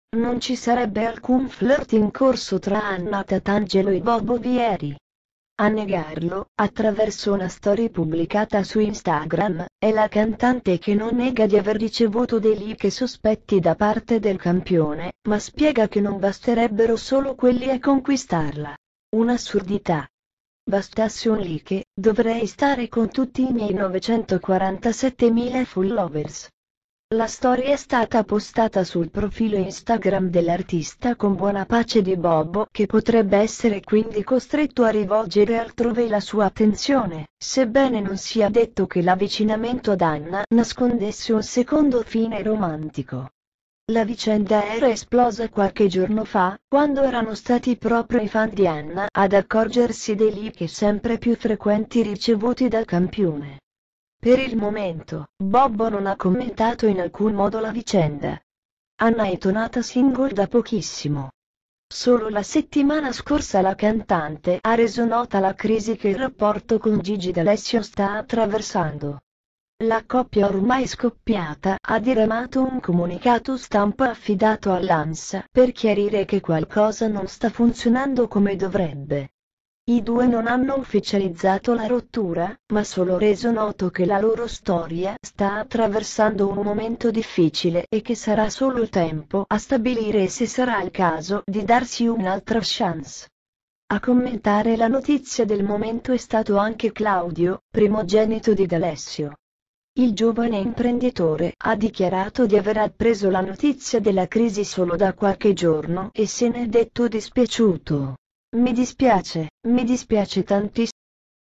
Anna Tatangelo nega il flirt con Bobo Vieri “Un’assurdità. Bastasse un like, starei con tutti”
Anna Tatangelo nega il flirt in corso con Bobo Vieri. La cantante bolla come un’assurdità il pettegolezzo che la vorrebbe vicina al latin lover: “Bastasse un like, dovrei stare con tutti i miei followers”.